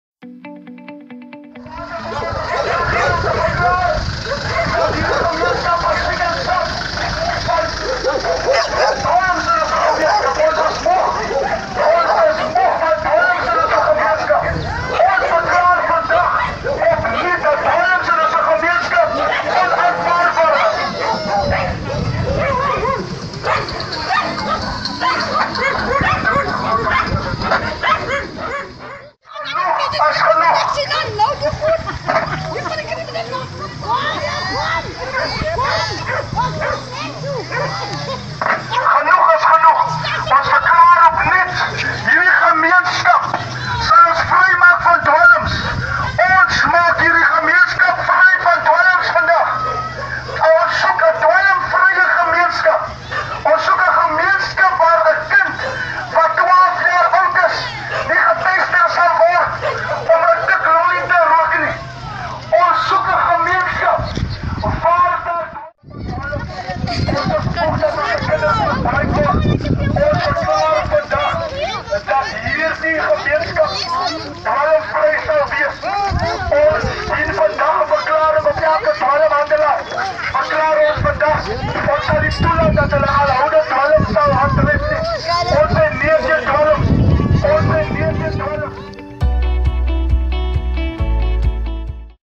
Albertinia staan op teen dwelms
Inwoners van Albertinia in die Suid-Kaap is keelvol vir dwelmmisbruik en -handel in die dorp, wat tot vele misdadige aksies lei. Op Vrydag 2 Augustus het hulle 'n vreedsame optog deur die strate gehou om hul stemme te laat hoor.

Video: Heinrich Fortuin